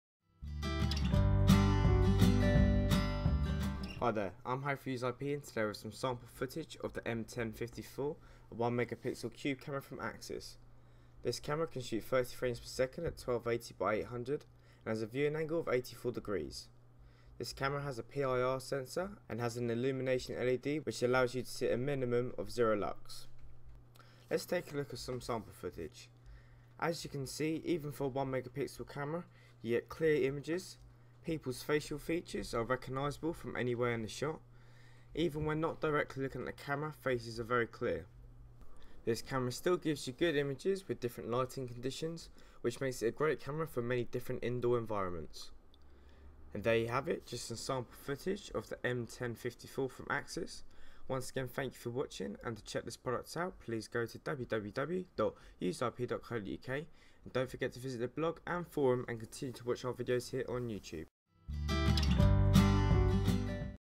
Axis M1054 Sample Footage video.
Today we have some sample footage of the M1054 a 1MP indoor cube camera from Axis.